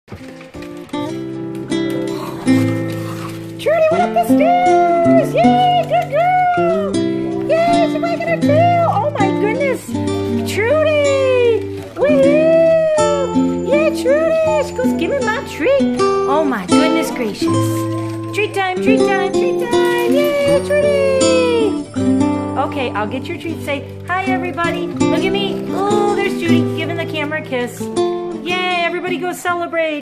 Trudy goes up stairs!
A huge leap! It took Trudy (7+yr puppymill survivor) 18 MONTHS to be brave enough to do it. I'm so proud...must...get...kleenex!